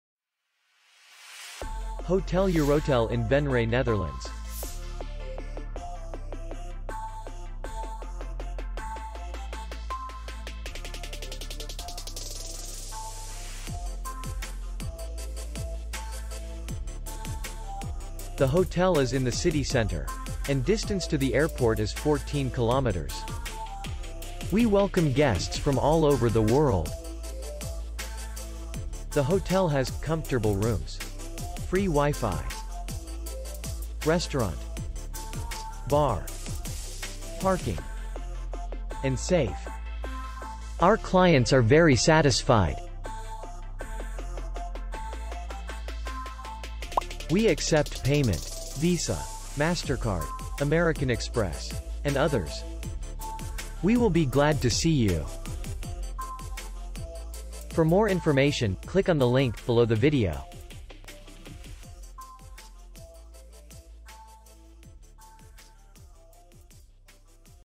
Hotel Eurotel hotel review | Hotels in Venray | Netherlands Hotels
Hotel Eurotel hotel video review. Hotels in Venray, european 3 stars hotels.